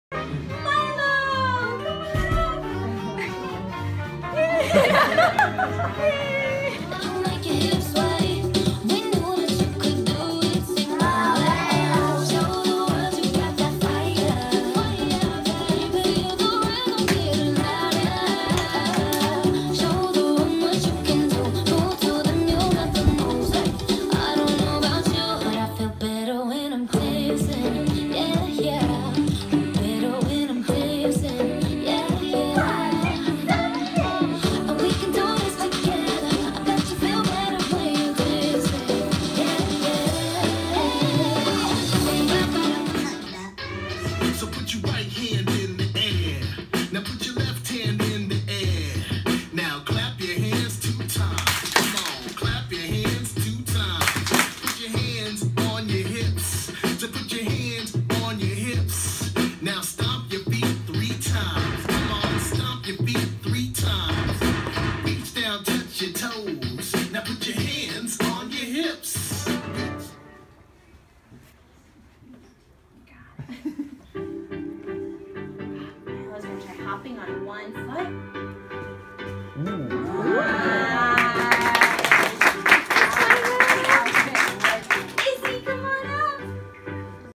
Milo's First dance recital
Happy feet recital at Smile's. May 19, 2016. Four years old.